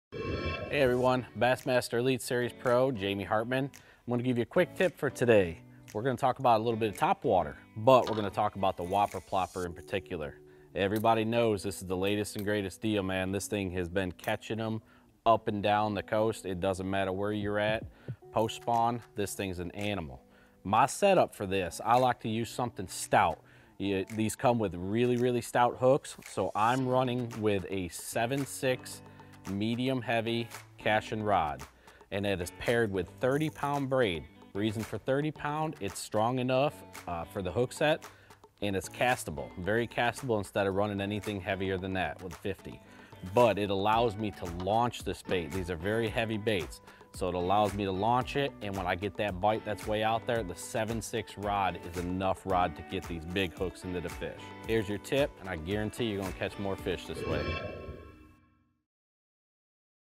Jamie Hartman's Whopper Plopper setup
Bassmaster Elite Series pro Jamie Hartman knows the power of a Whopper Plopper topwater lure when fish move into the post spawn phase of the year.